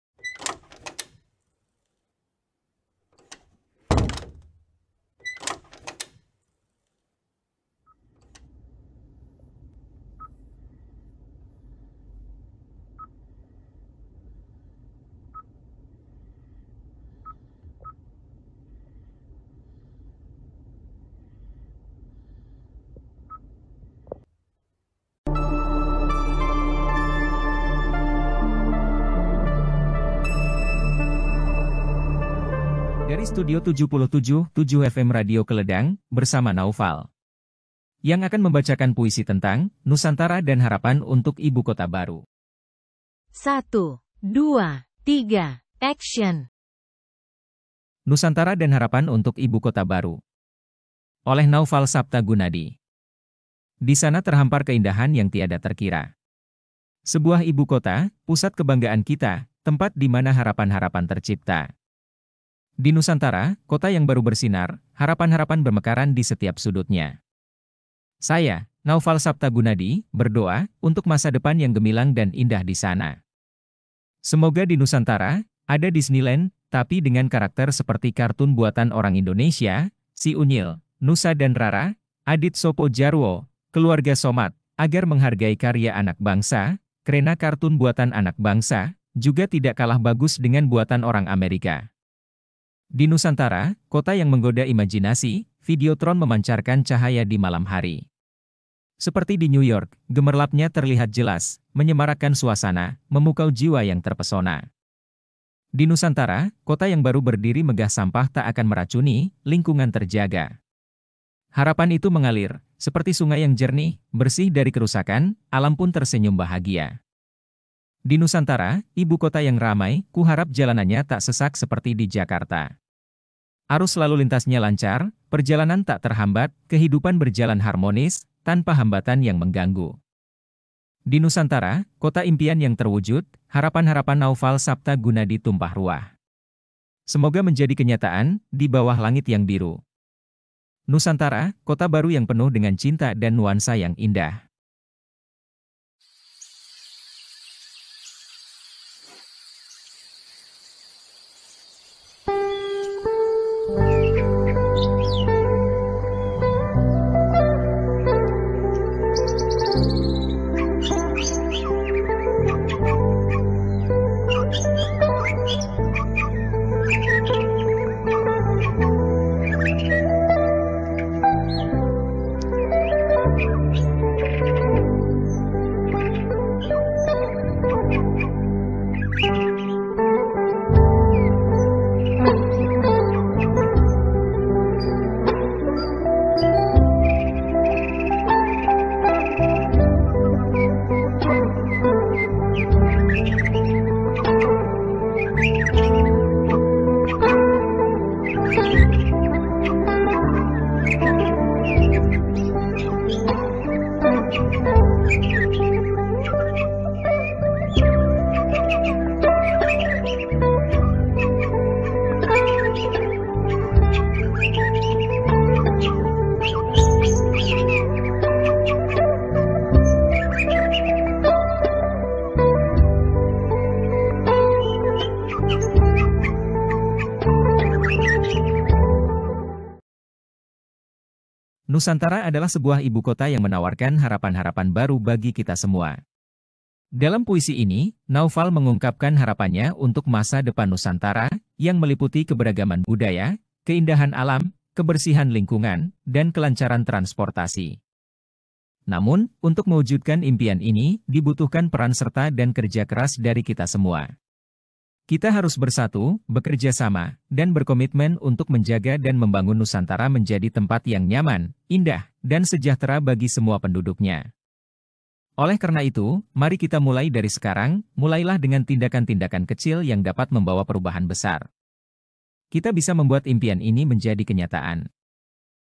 77 7 Keledang Puisi Tentang Nusantara dan harapan untuk ibu kota baru
Perhatian ... jangan bandingkan puisi ini dengan puisinya Bapak Chairil Anwar atau tokoh penulis puisi lainnya ya.
Soalnya ini puisi buatan seorang Naufal Yang sangat mengharapkan cepat terselesaikannya IKN.
MERDEKA !

sumber video/foto/musik